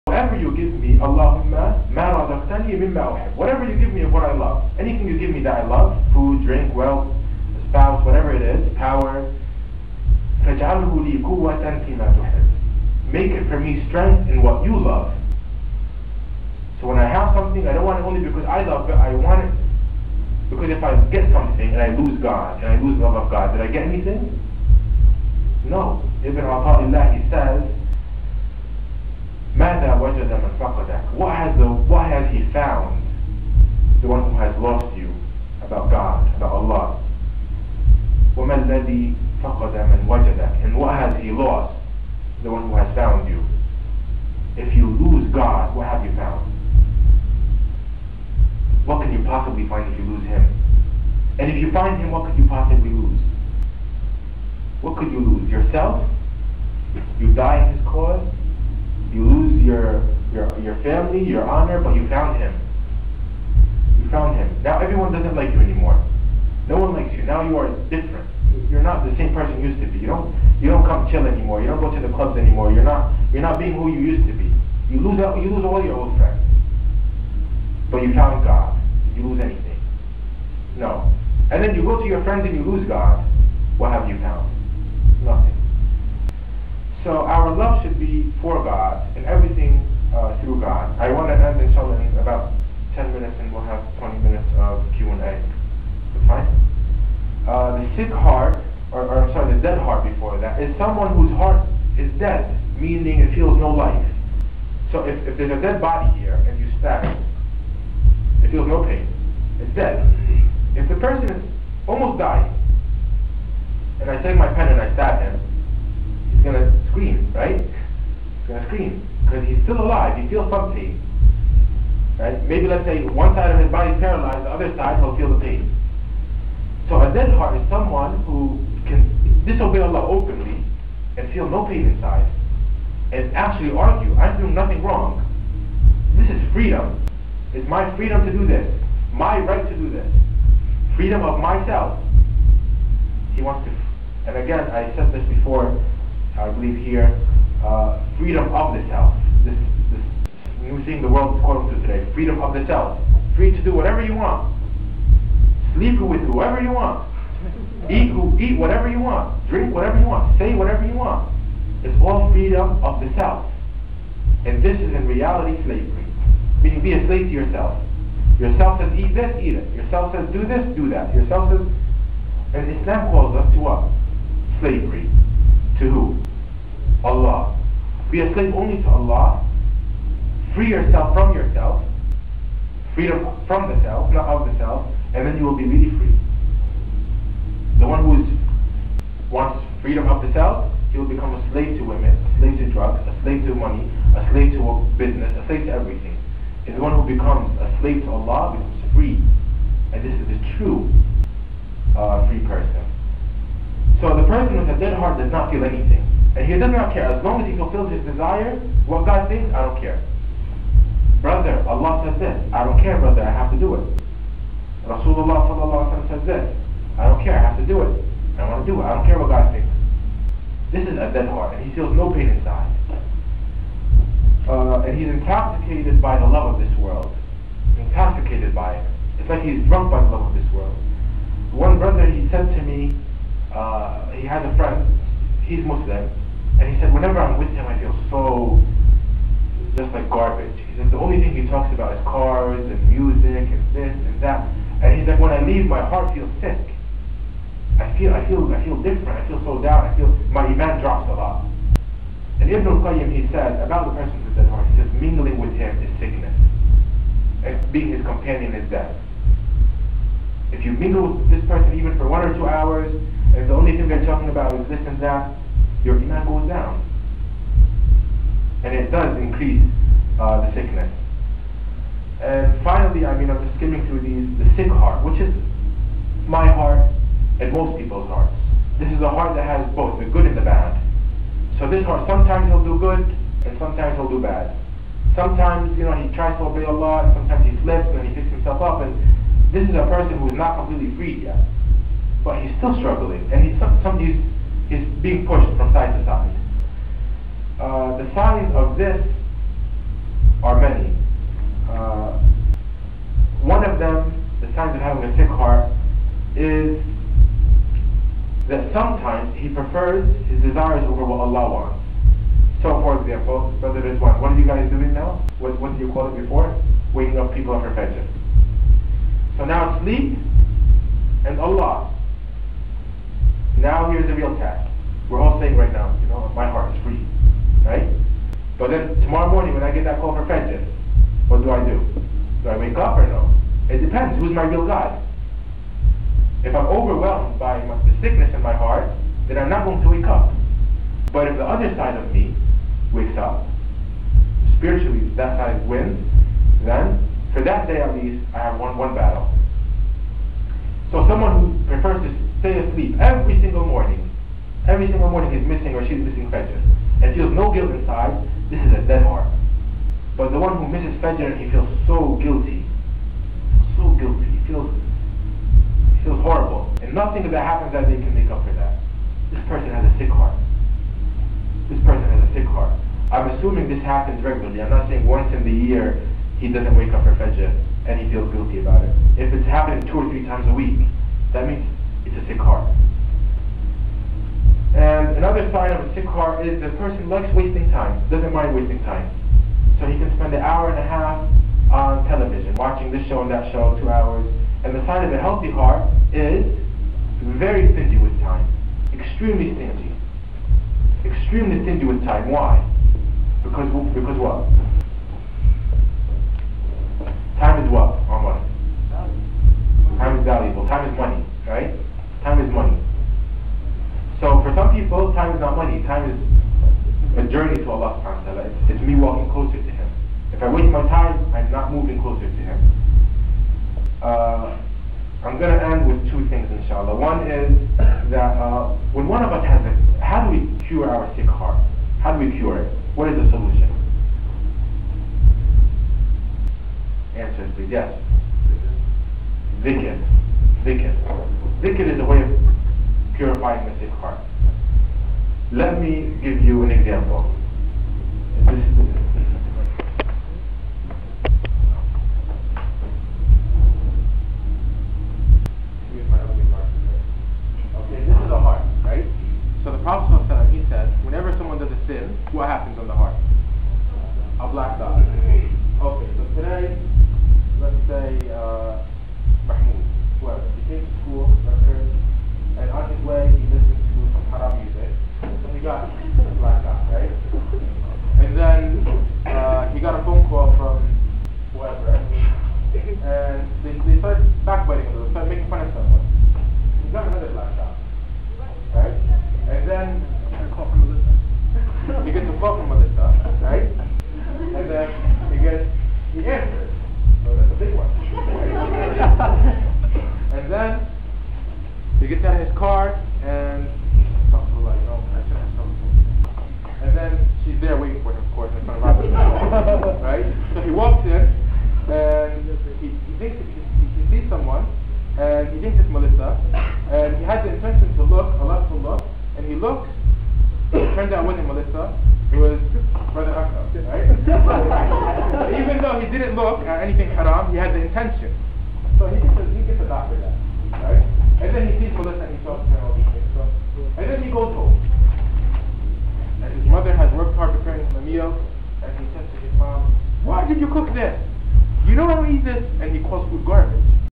Diseases of the Heart (4-6)
Brother Emad Hamdeh discusses the symptoms and cures of a diseased heart.